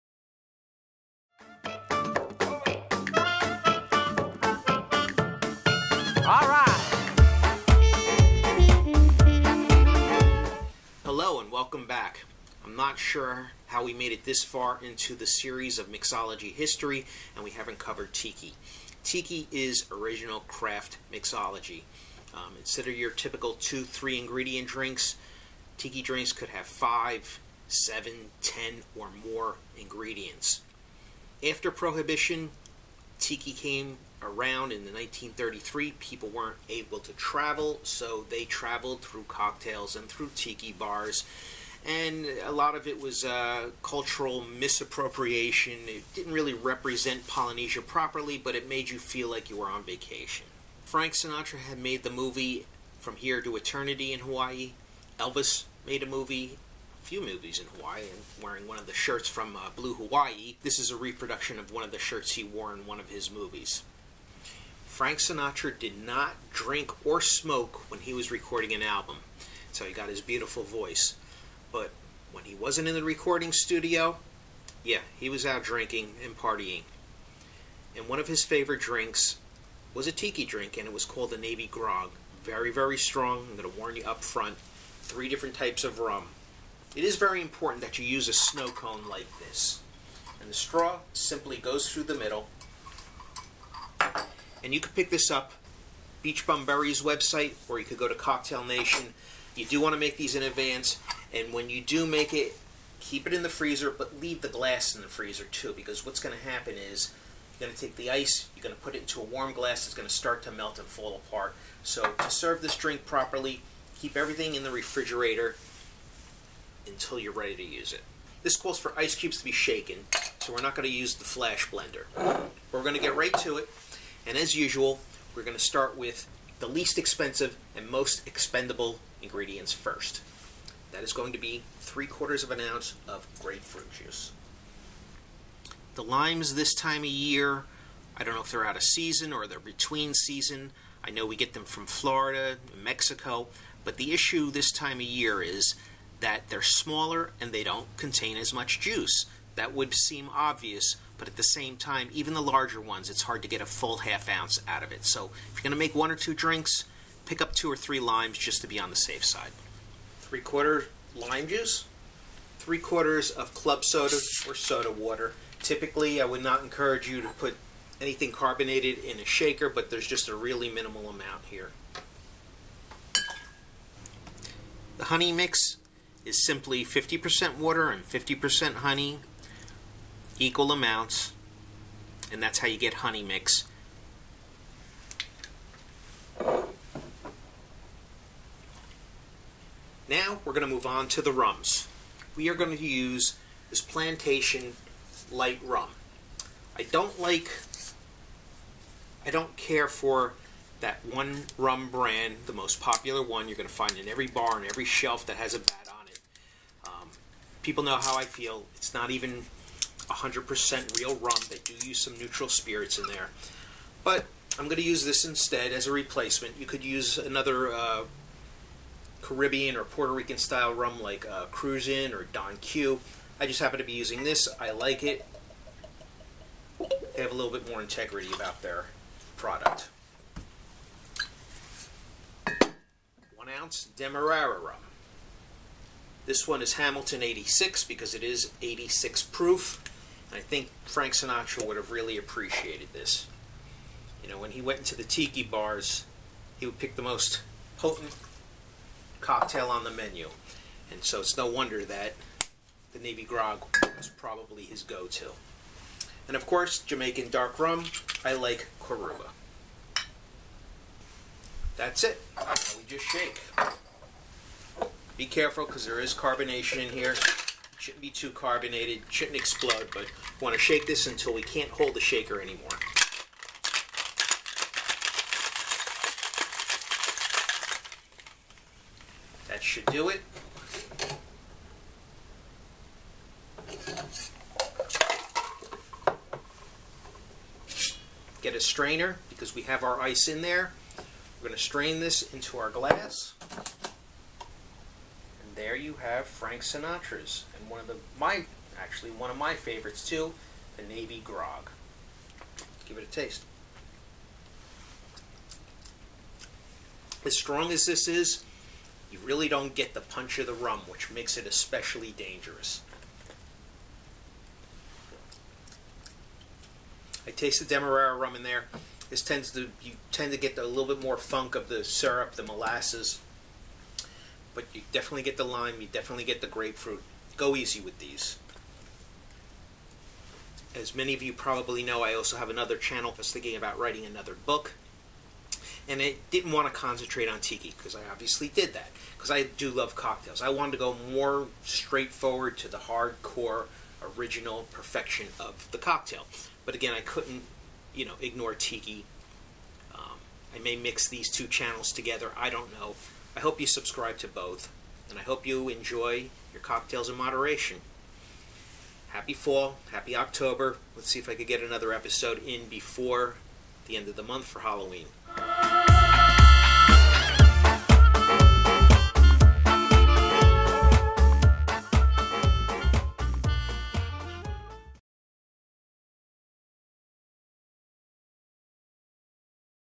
Mixology History #7 - Navy Grog
Mixology History #7 - The Navy Grog, Frank Sinatra's favorite tiki drink!